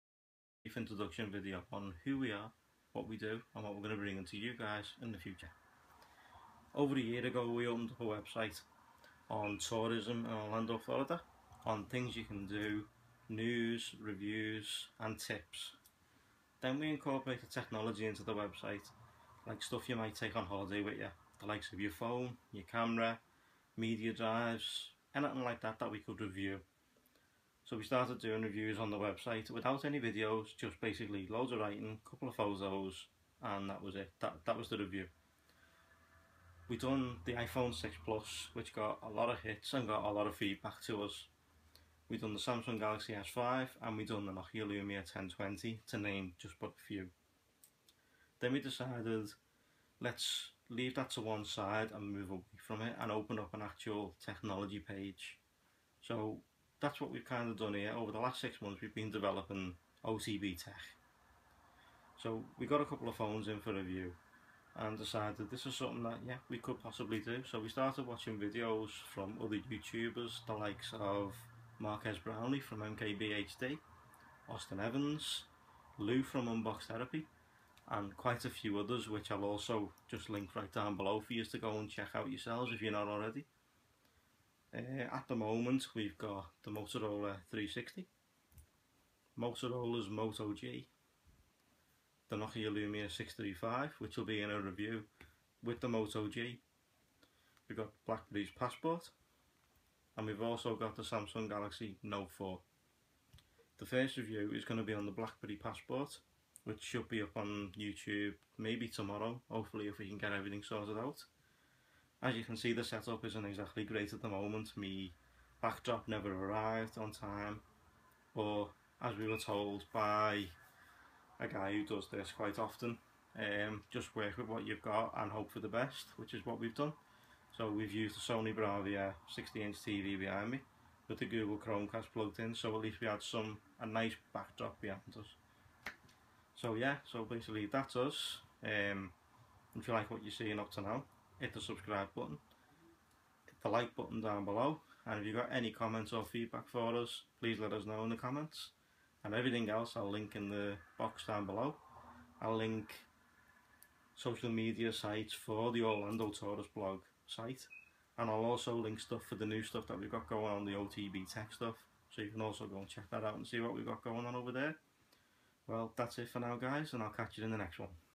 My First Time In Front Of The Camera
This is our first video as a tech channel, normally we only cover orlando Florida.
We decided to do this 6 months ago as the video will explain.

Our website for everything we do is feel free to check us out.

we wanna say a special thanks to all those that gave there advice.

Watching these guys do what they do really inspired me to do this.

This clip only took 15 takes, yikes!!!